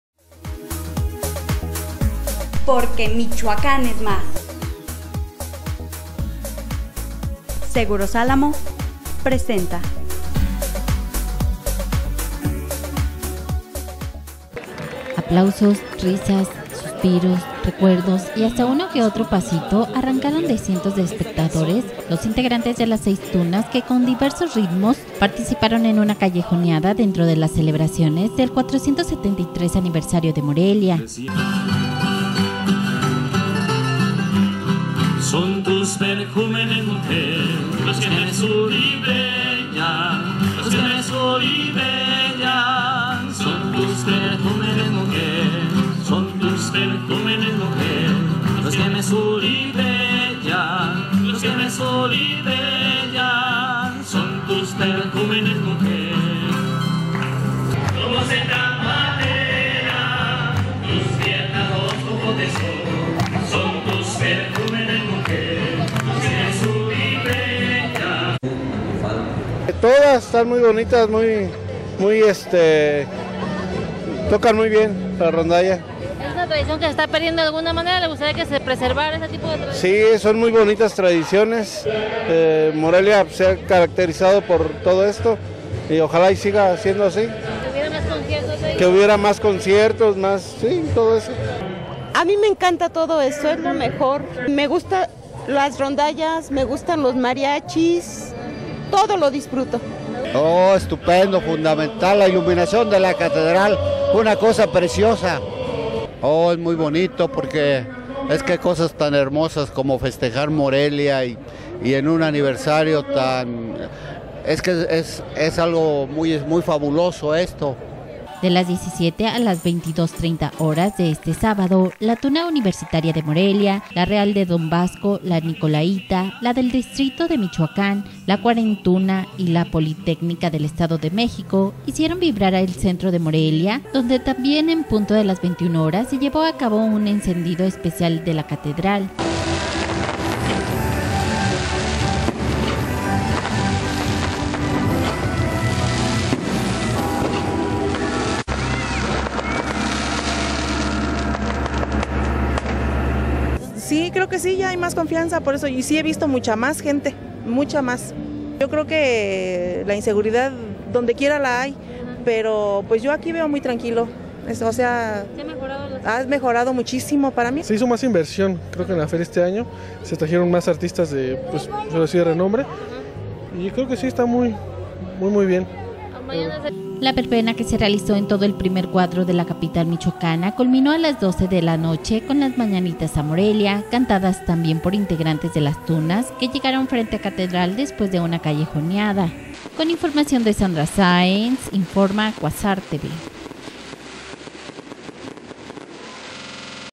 Tunas le dan serenata a Morelia en su aniversario
Aplausos, risas, recuerdos y hasta uno que otro pasito, arrancaron cientos de  espectadores los integrantes de las seis Tunas que con diversos ritmos participaron en una callejoneada dentro de las celebraciones del 473 aniversario de Morelia...